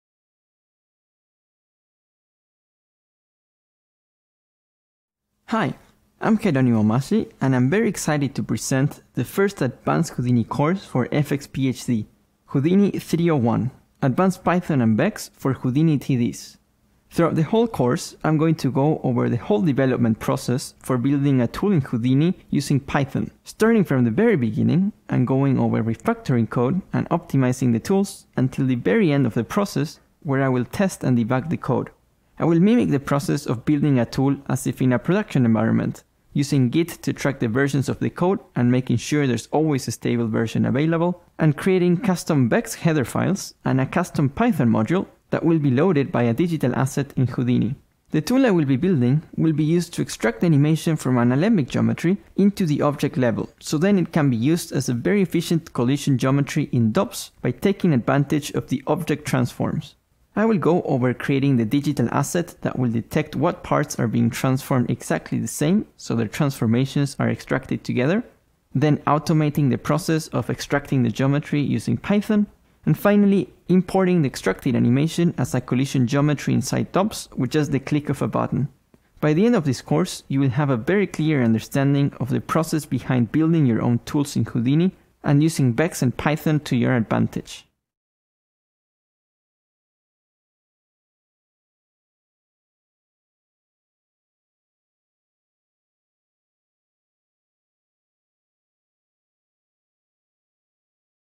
HOU301 - Advanced VEX & Python for Houdini TDs
This course, taught by Jeronimo Maggi, builds on the fundamentals taught in Introduction to Houdini VEX & Python (HOU217), focusing on creating a tool using mostly Python and some VEX. We start from scratch and slowly add one feature on top of the other as the needs come along, ending up learning how to debug and test our code.